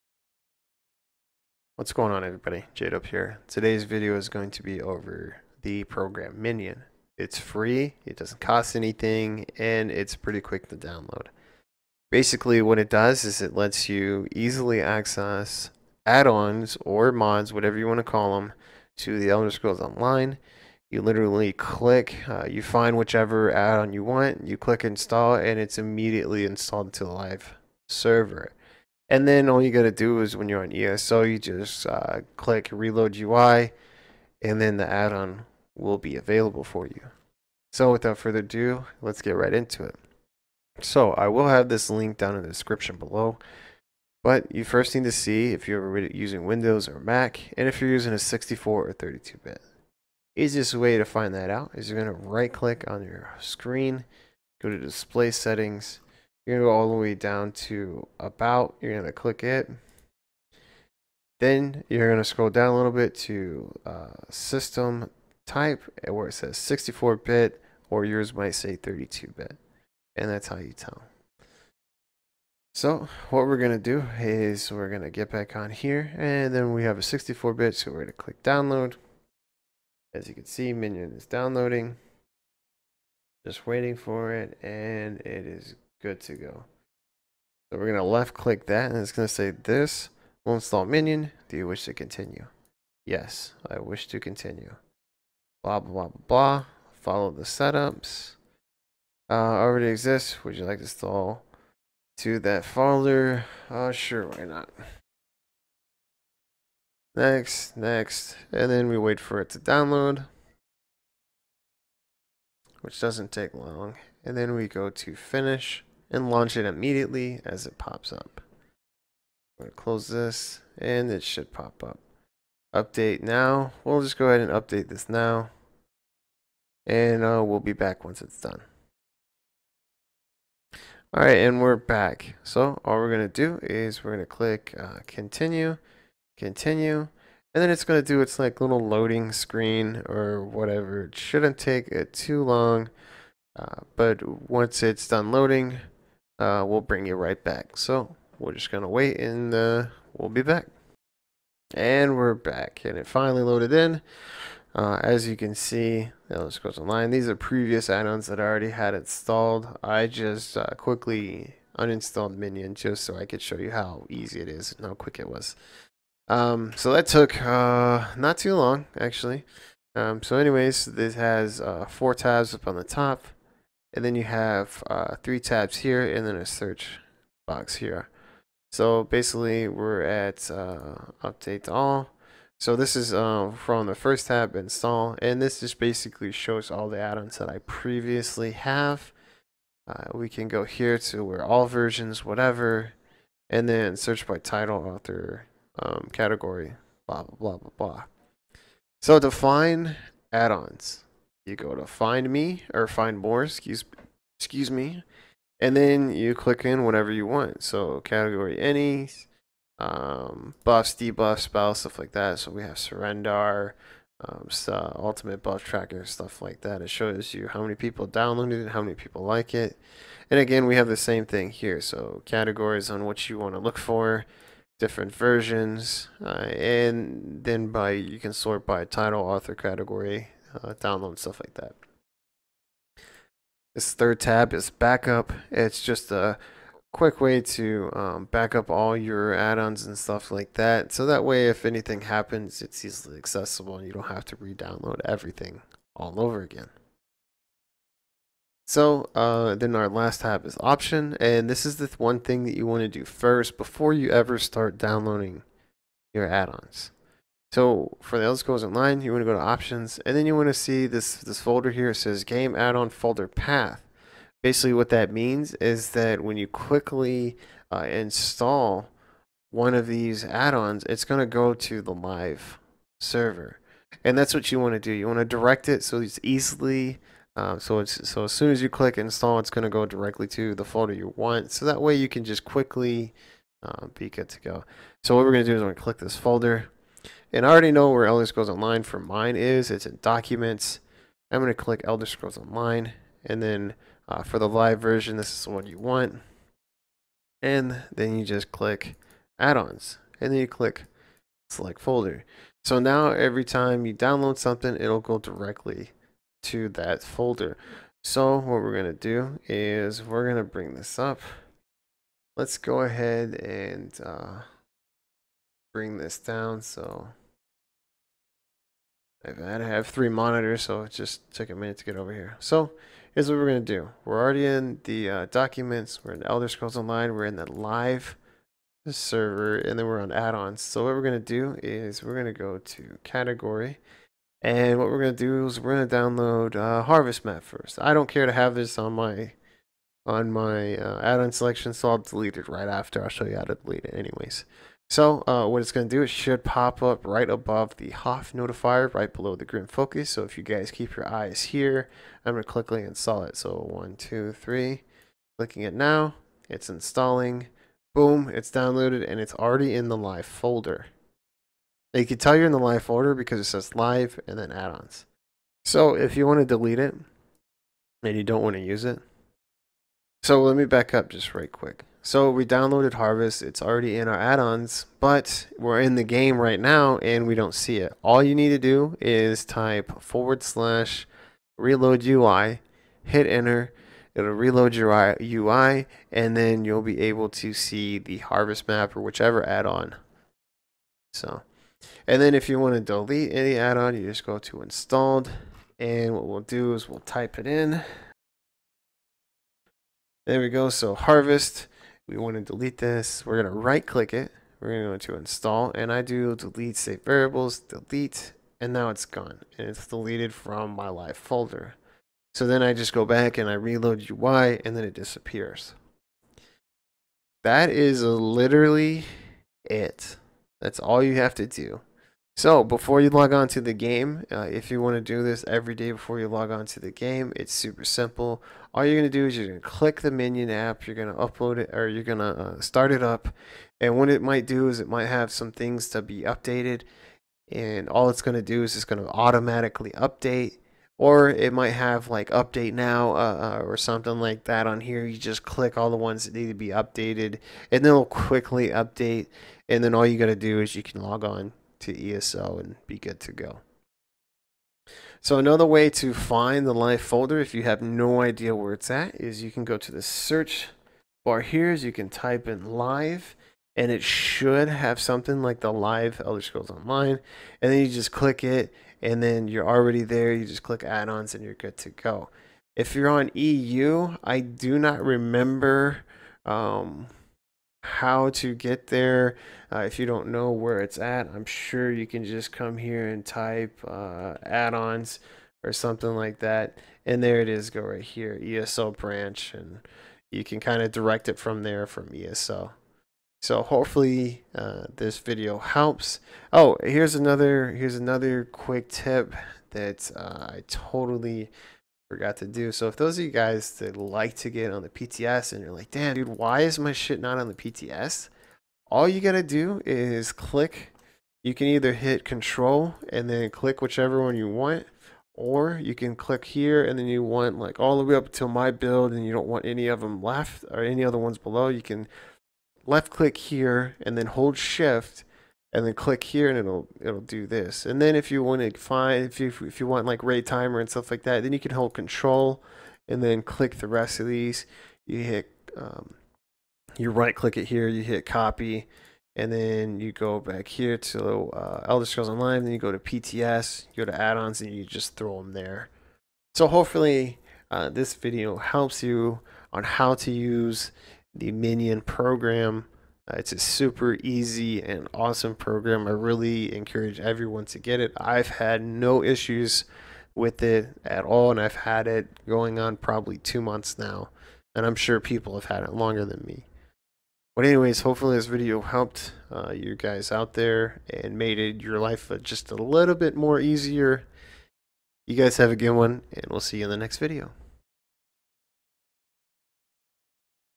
ESO - How to use Minion!
This video shows you how to use Minion. Minion is an awesome program that lets you easily download add-ons to the live folders of the Elder Scrolls Online.